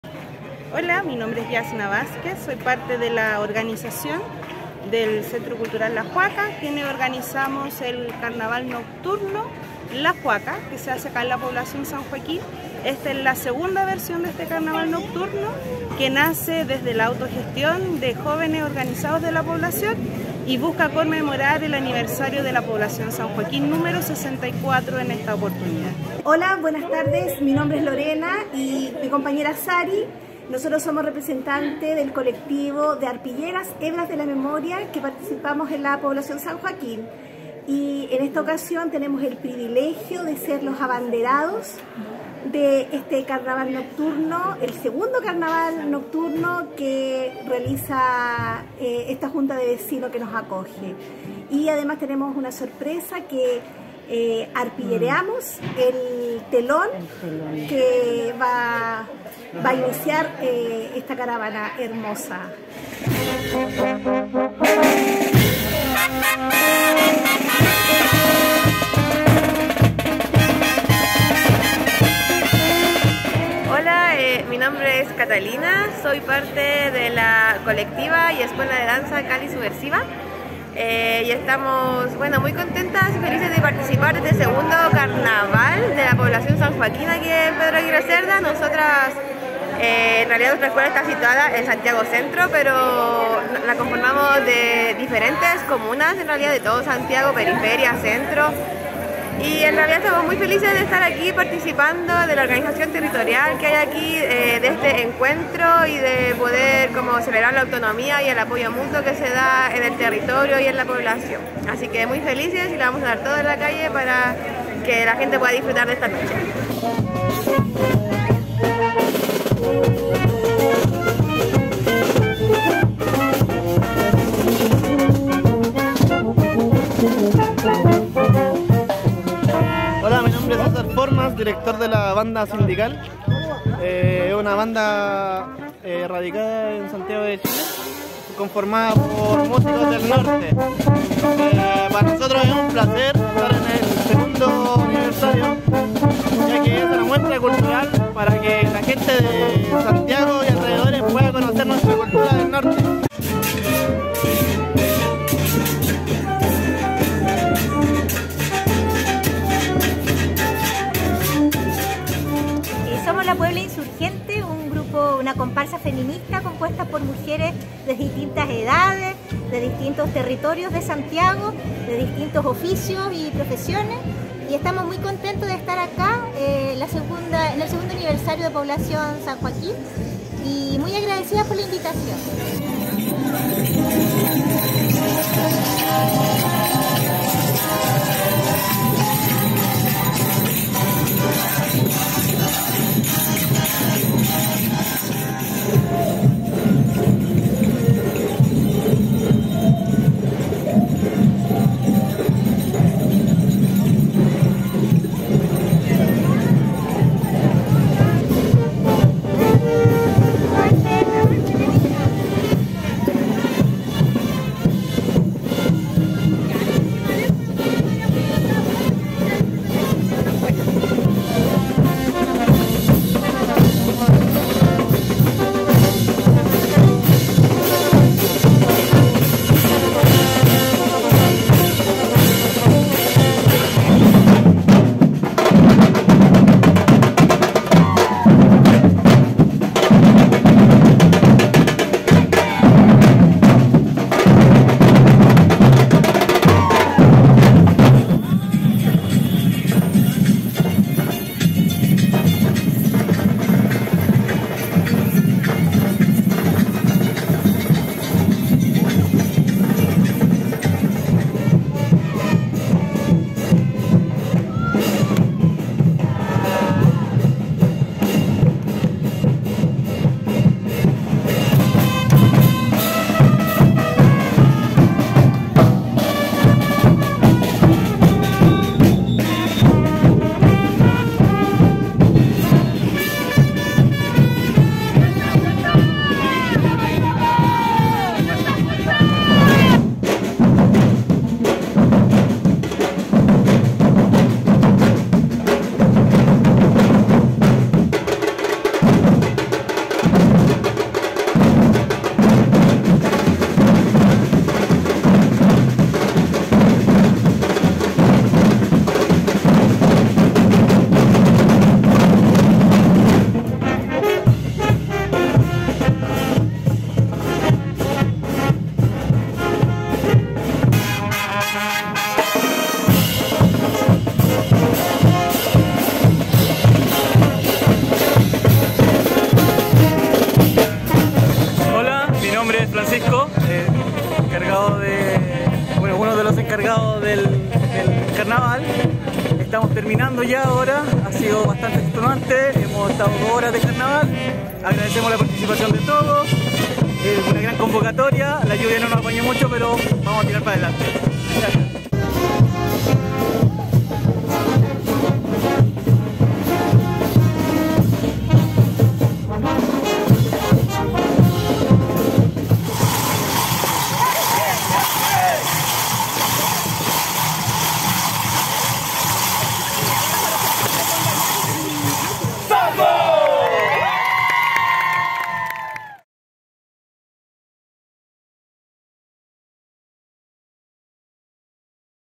2º Carnaval Nocturno San Joaquín
Con música y danzas se conmemoró  el 2º Carnaval Nocturno,  "La Juaca",  en el marco del aniversario nº 64 de Población San Joaquín.

Primer canal de televisión Popular y Comunitario de Chile